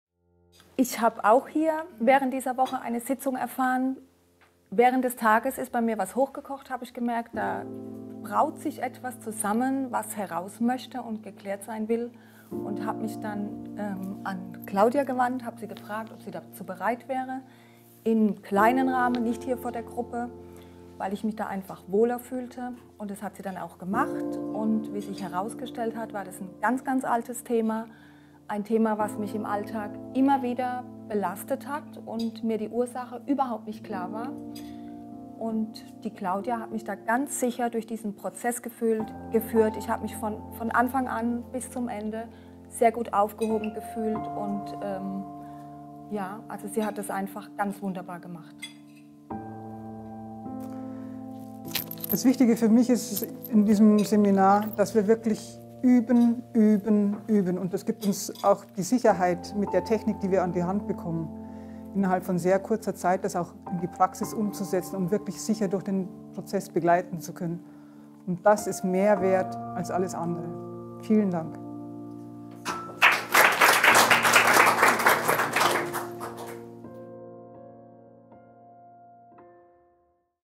Erweitere deine Hypnose-Fähigkeiten mit dem einzigartigen Yager-Code und lerne eine der effektivsten Therapiemethoden für die Behandlung von Beschwerden deiner Klienten und Patienten kennen.